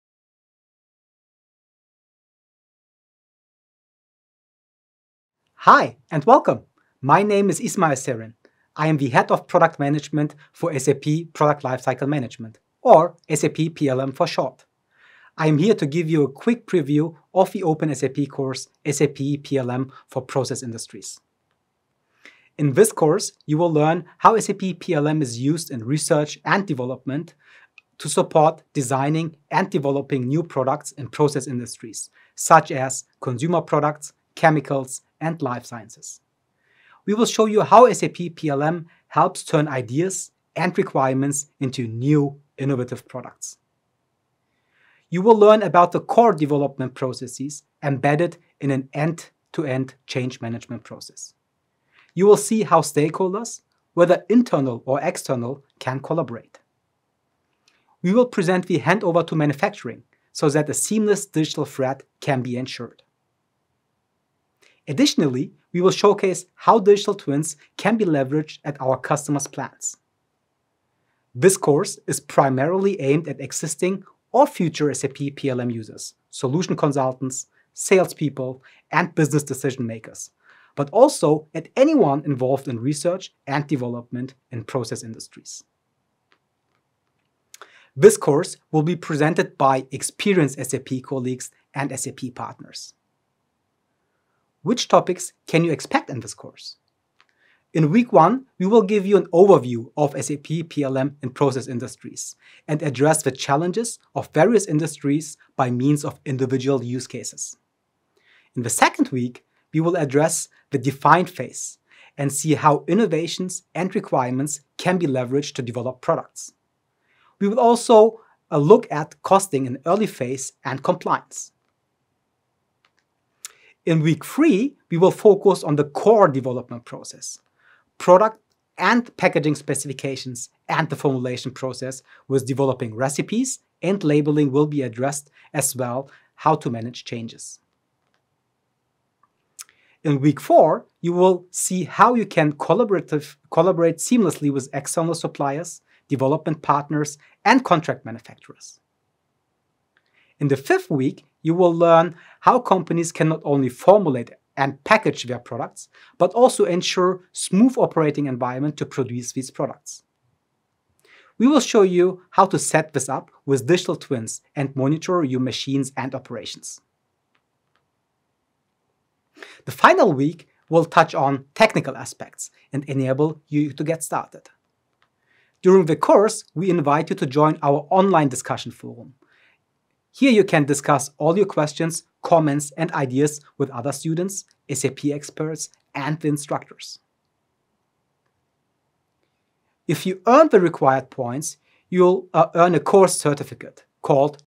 SAP PLM for Process Industries (Update Q2/2023) - openSAP Free Crouse (Teaser)
Join this free online course to learn about SAP Product Lifecycle Management for process industries and find out how it helps you turn your ideas into new innovative products by exploring the whole product development process.
In this course, you’ll learn how teams can work together efficiently from idea to market with a clear focus on the DEFINE and DEVELOP stages. You’ll understand how to identify requirements and map these to future products. You’ll also learn how to identify cost drivers early on, and how the efficient management of R&D projects helps to see the workload ahead. In addition, the course will guide you through the process of creating the detailed product specification, the development of compliant recipes, and finally, the handover to manufacturing. This course will show you how to enrich product development processes using intelligent and collaborative tools to formulate improved or brand-new products in process industries.

Course Content
Week 1: Introducing SAP PLM for Process Industries
Week 2: Digitize the Define Phase
Week 3: Streamline the Development Process
Week 4: Collaborate Across Enterprise Boundaries
Week 5: Deliver and Operate in a Well-Maintained Plant
Week 6: Get Started and Technical Foundation
Week 7: Final Exam